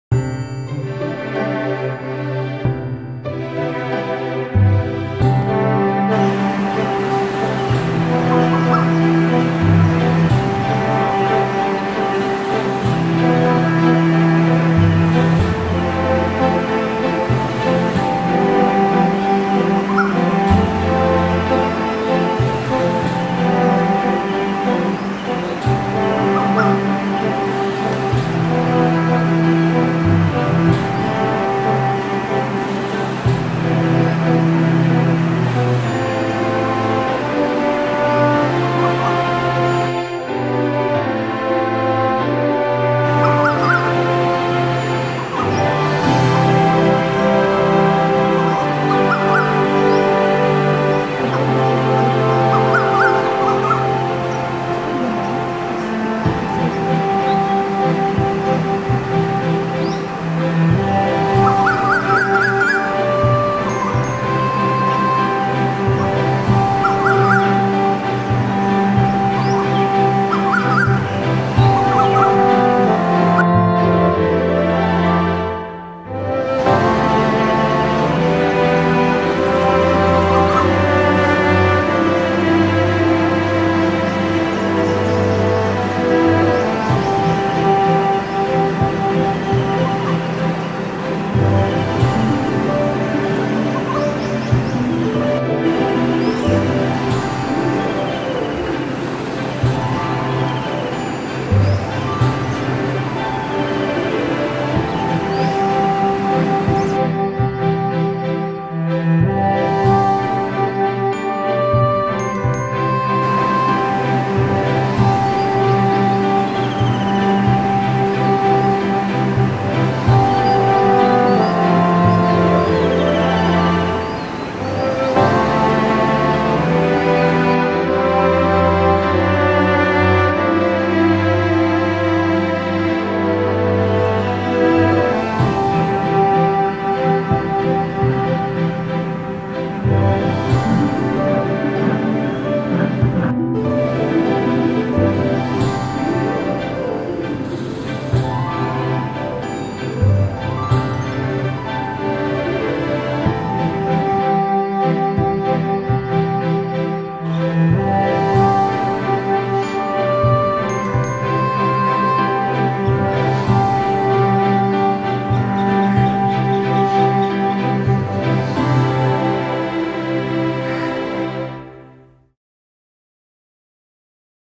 Asian Koel
Asian koel family in my neighbourhood...